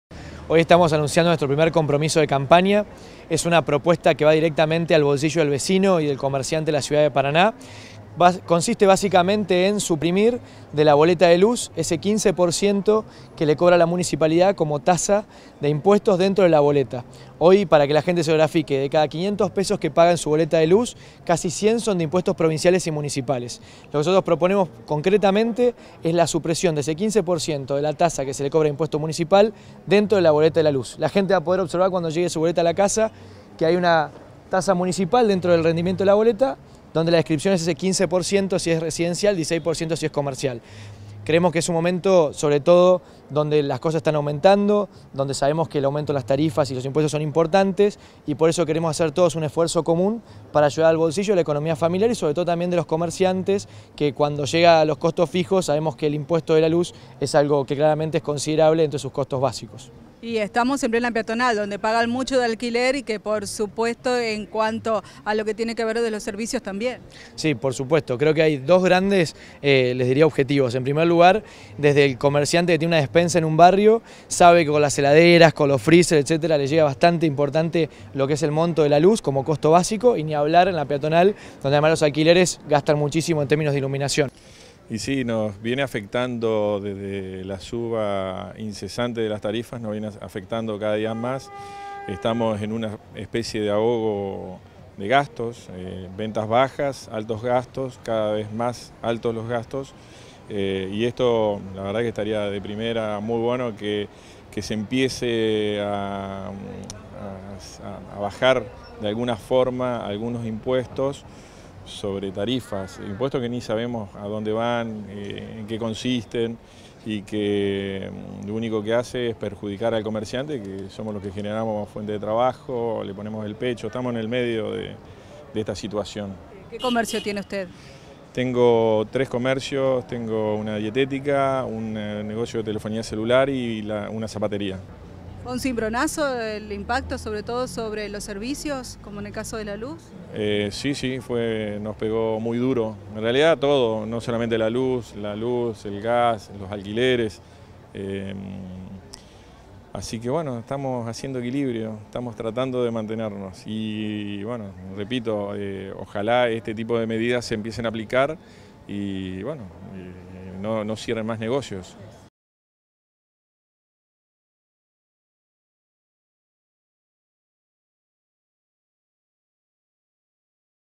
GAINZA
GAINZA PROPUSO LA ELIMINACIÓN DEL IMPUESTO MUNICIPAL A LA BOLETA DE LUZ
"PROPONEMOS LA SUPRESIÓN DE ESE 15% DE LA TASA QUE LE COBRA POR IMPUESTO MUNICIPAL DENTRO DE LA BOLETA DE LA LUZ", ANUNCIÓ A ELONCE TV EL PRECANDIDATO A LA INTENDENCIA DE PARANÁ POR CAMBIEMOS, EMANUEL GAINZA.